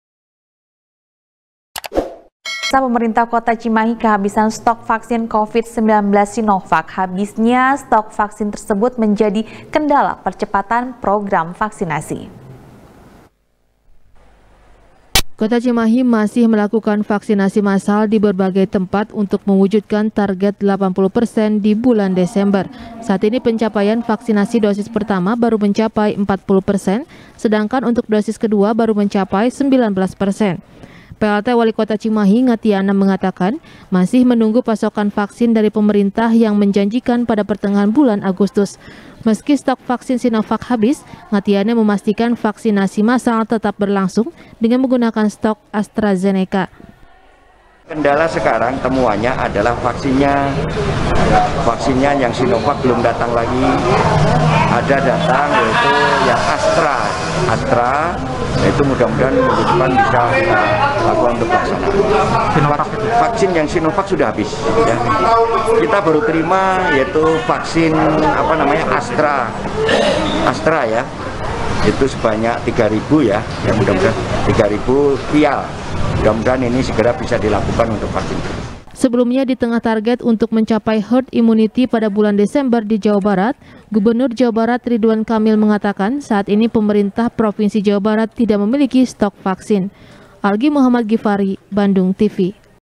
Stok Vaksin Sinovac Di Cimahi Habis | SBR | BANDUNGTV
SBR | BANDUNGTV | KRISANTI | Pemerintah Kota Cimahi Kehabisan Stok Vaksin Covid 19, Sinovac. Habisnya Stok Vaksin Tersebut, Menjadi Kendala Percepatan Program Vaksinasi.